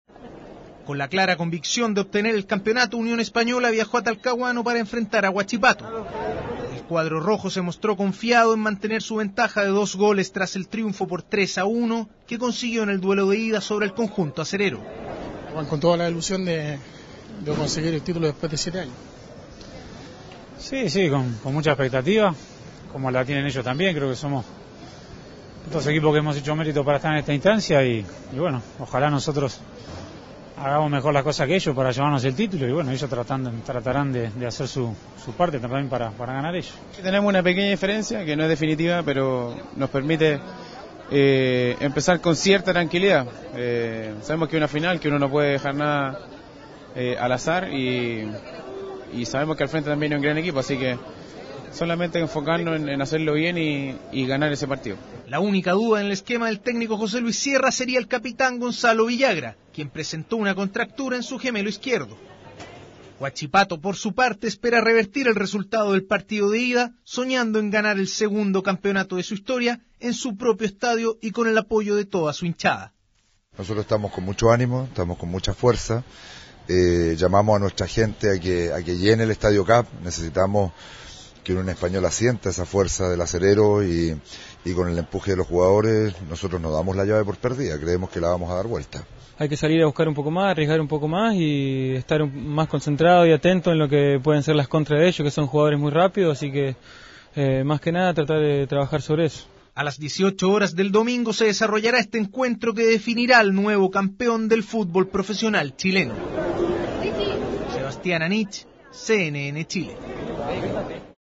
La previa de la final del Torneo de Clausura entre Unión Española y Huachipato
Este domingo a las 18 horas, rojos y acereros definirán al nuevo campeón del fútbol chileno.